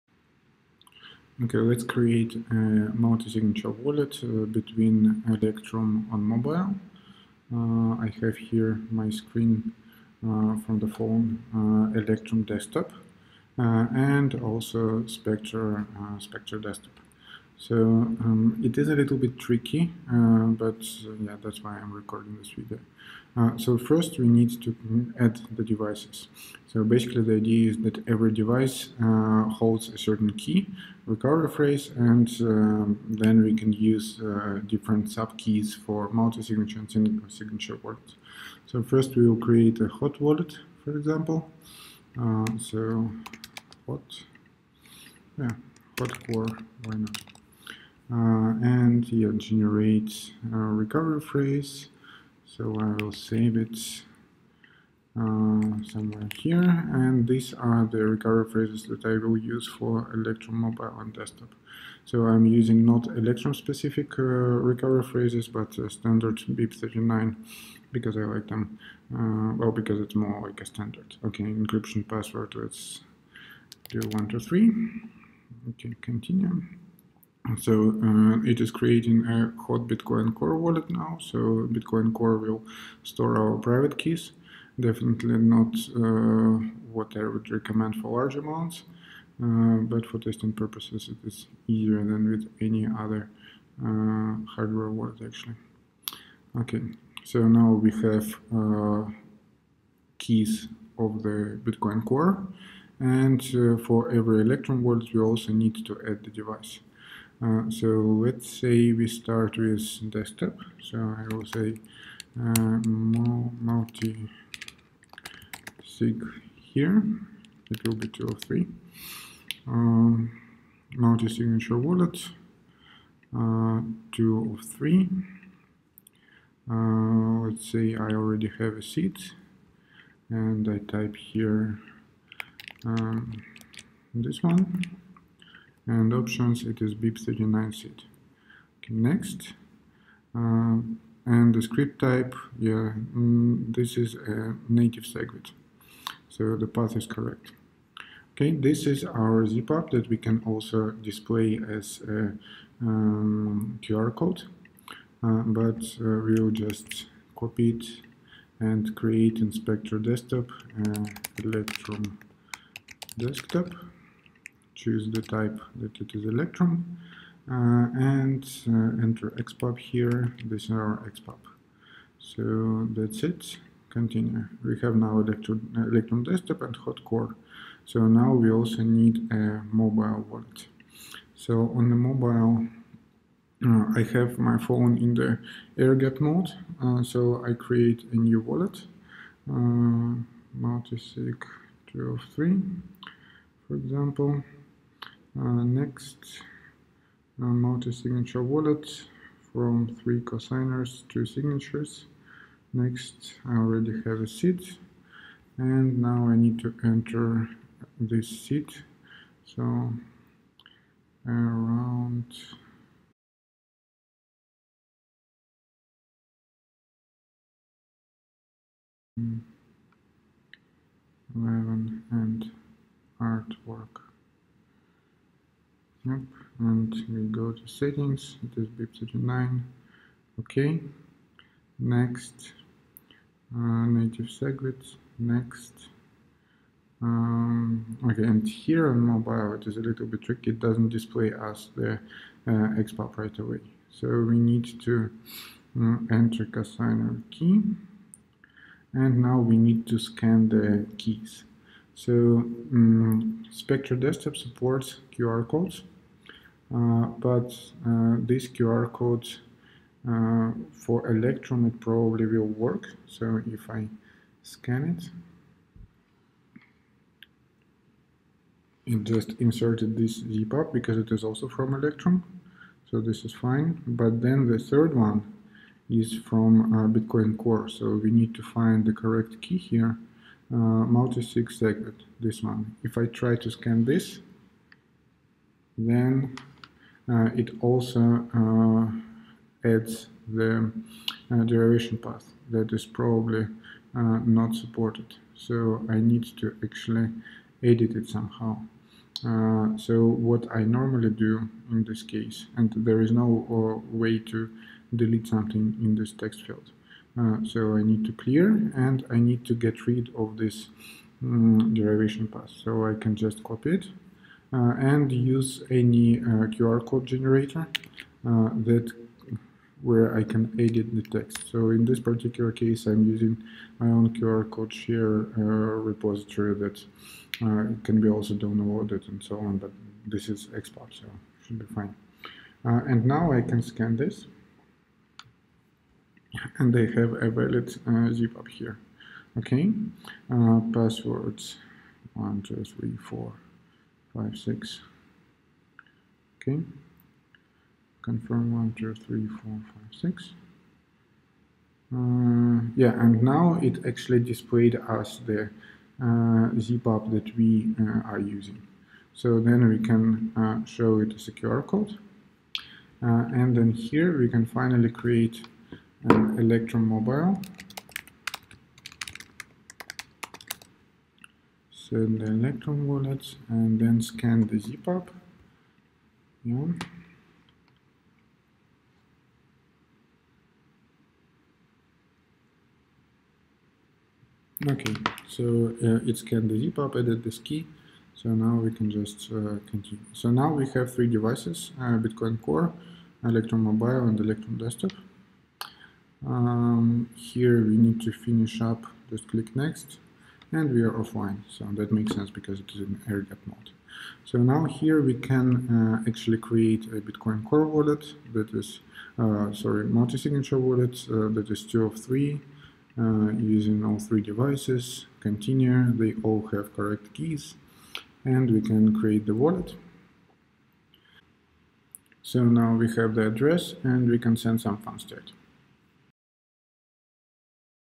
Setting up a multisig wallet with Specter and Electrum
Electrum in airgapped mode can be used as one of the devices in Specter-Desktop. In this video I show how to set up a 2-of-3 multisig wallet between Specter-Desktop, Electrum on mobile and Electrum on desktop